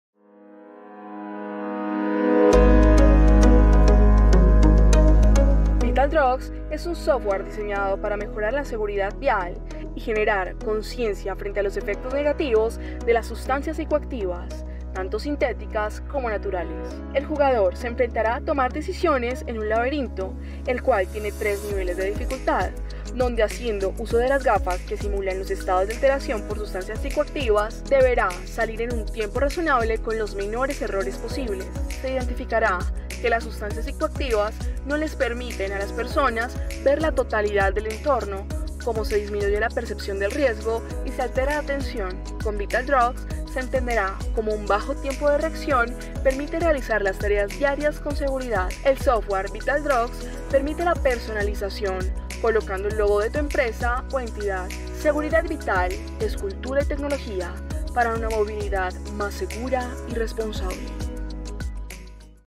Software Seguridad Vial Vital Drugs
Vital Drugs es un software diseñado para mejorar la Seguridad Vial y generar conciencia frente a los efectos negativos de las sustancias psicoactivas, tanto sintéticas como naturales. El jugador se enfrentará a tomar decisiones en un laberinto, el cual tiene tres niveles de dificultad, donde haciendo uso de las gafas que simulan estados de alteración por sustancias psicoactivas, deberá salir en un tiempo razonable y con los menores errores posibles.
Se identificará que las sustancias psicoactivas no les permiten a las personas ver la totalidad del entorno, cómo se disminuye la percepción del riesgo y se altera la atención. Con Vital Drugs se podrá entender como un bajo tiempo de reacción facilita realizar las tareas diarias con seguridad.
El software Vital Drugs permite la personalización colocando el logo de tu empresa o entidad.
¡Seguridad vital es cultura y tecnología para una movilidad más segura y responsable!